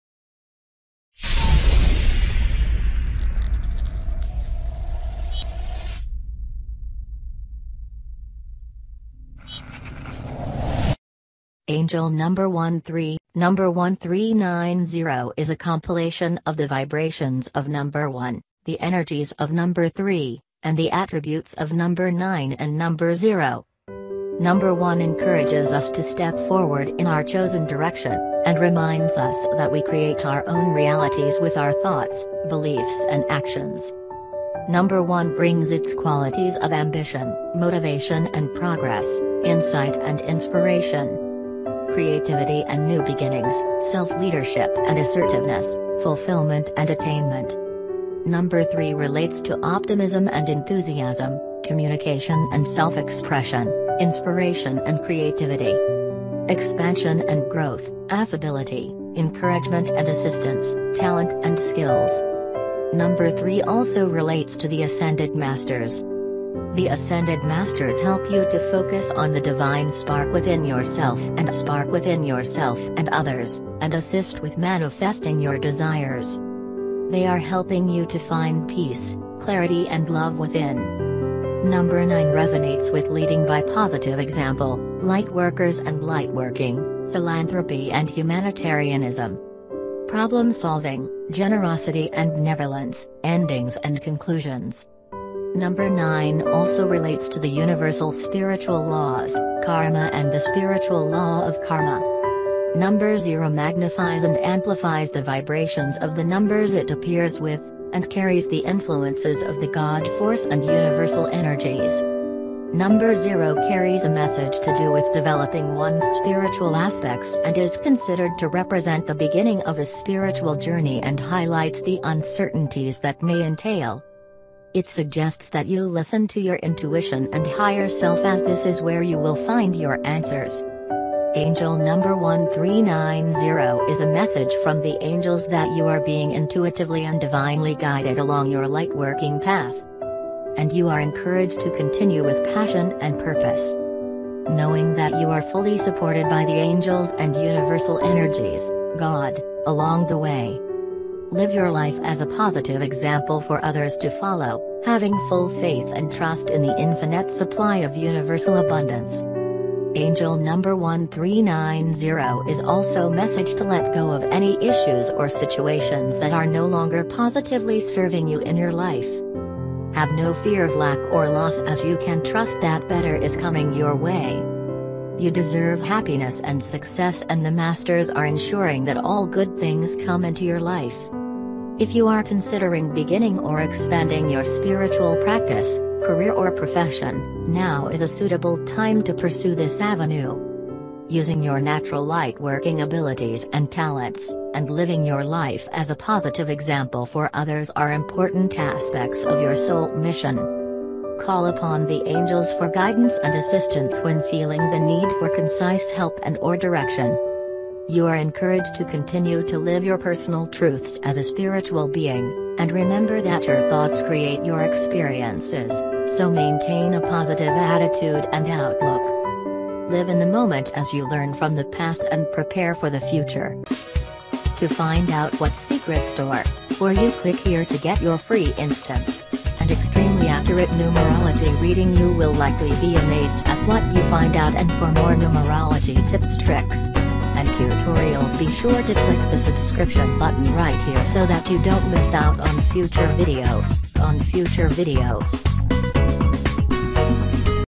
🔴 Message From Your Angels - Angel Number 1390 ✅ Numerology Box
ANGEL NUMBER 1390 - Universe Signal

Number 1390_meaning is a compilation of the vibrations of number 1, the energies of number 3, and the attributes of number 9 and number 0. Number 1 encourages us to step forward in our chosen direction, and reminds us that we create our own realities with our thoughts, beliefs and actions. Number 1 brings its qualities of ambition, motivation and progress, insight and inspiration, creativity and new beginnings, self-leadership and assertiveness, fulfilment and attainment. Number 3 relates to optimism and enthusiasm, communication and self-expression, inspiration and creativity, expansion and growth, affability, encouragement and assistance, talent and skills. Number 3 also relates to the Ascended Masters. The Ascended Masters help you to focus on the Divine spark within yourself and others, and assist with manifesting your desires. They are helping you to find peace, clarity and love within. Number 9 resonates with leading by positive example, lightworkers and lightworking, philanthropy and humanitarianism, problem solving, generosity and benevolence, endings and conclusions. Number 9 also relates to the Universal Spiritual Laws, karma and the Spiritual Law of Karma. Number 0 magnifies and amplifies the vibrations of the numbers it appears with, and carries the influences of the God force and Universal Energies. Number 0 carries a message to do with developing one’s spiritual aspects and is considered to represent the beginning of a spiritual journey and highlights the uncertainties that may entail. It suggests that you listen to your intuition and higher-self as this is where you will find your answers.

Angel Number 1390 is a message from the angels that you are being intuitively and Divinely guided along your lightworking path, and you are encouraged to continue with passion and purpose, knowing that you are fully supported by the angels and Universal Energies (God) along the way. Live your life as a positive example for others to follow, having full faith and trust in the infinite supply of Universal abundance.

angel_number_1390 is also message to let go of any issues or situations that are no longer positively serving you in your life. Have no fear of lack or loss as you can trust that ‘better’ is coming your way. You deserve happiness and success and the Masters are ensuring that all good things come into your life.

If you are considering beginning or expanding your spiritual practice, career or profession, now is a suitable time to pursue this avenue. Using your natural lightworking abilities and talents, and living your life as a positive example for others are important aspects of your soul mission. Call upon the angels for guidance and assistance when feeling the need for concise help and/or direction. You are encouraged to continue to live your personal truths as a spiritual being, and remember that your thoughts create your experiences, so maintain a positive attitude and outlook.

Live in the moment as you learn from the past and prepare for the future.